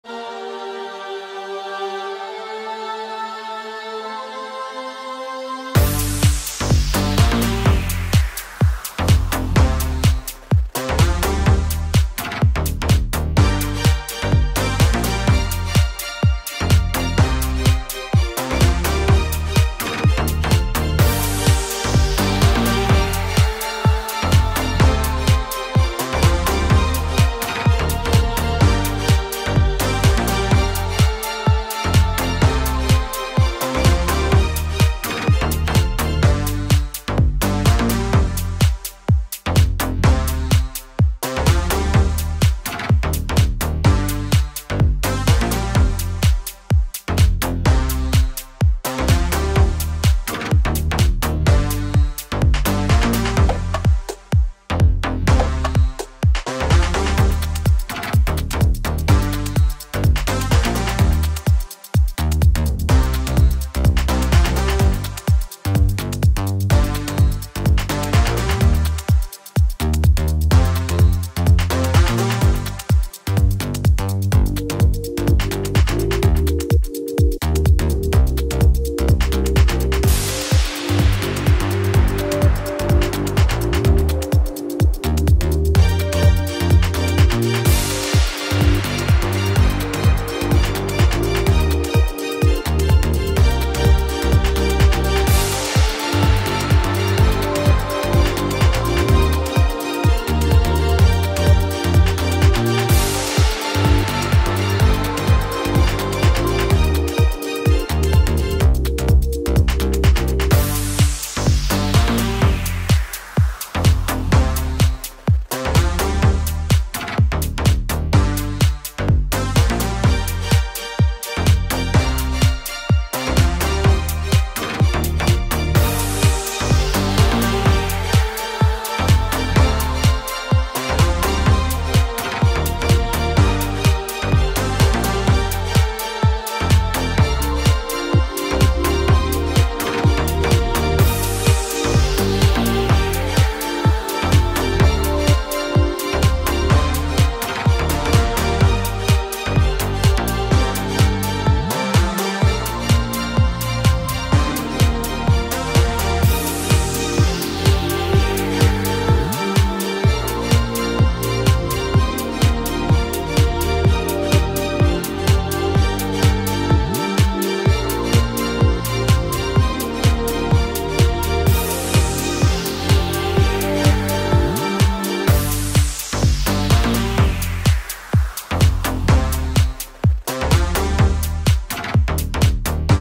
Blackpool Fireworks Poland
Great display from Poland . Music created by my self